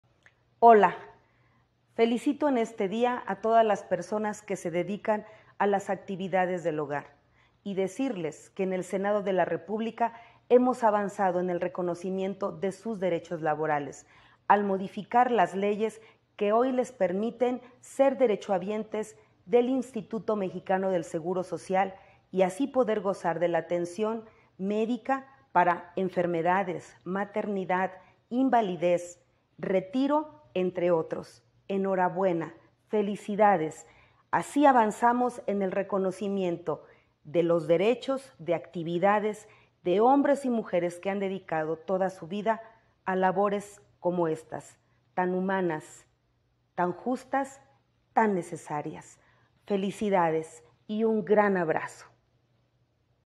Mensaje por el Día Internacional De Las Personas Trabajadoras Del Hogar
Senadora Ana Lilia Rivera - Hoy en el Día Internacional De Las Personas Trabajadoras Del Hogar, quiero felicitar todas aquellas personas que se dedican a esta invaluable actividad, y reiterarles mi compromiso para seguir legislando por su bienestar.  ¡Enhorabuena para todxs!